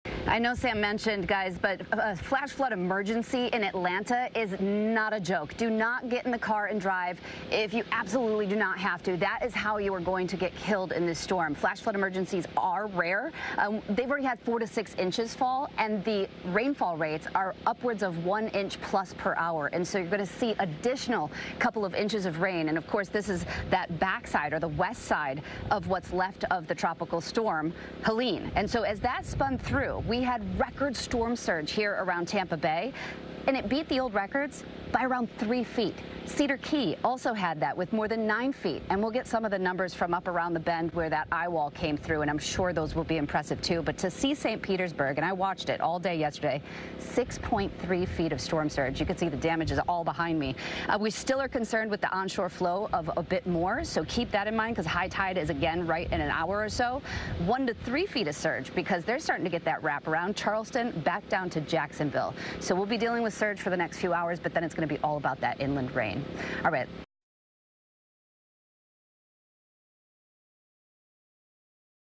Hurricane Helene brings dangerous storm surge
Forecasters had warned that the enormous system could create a nightmare storm surge across much of the southeastern U.S.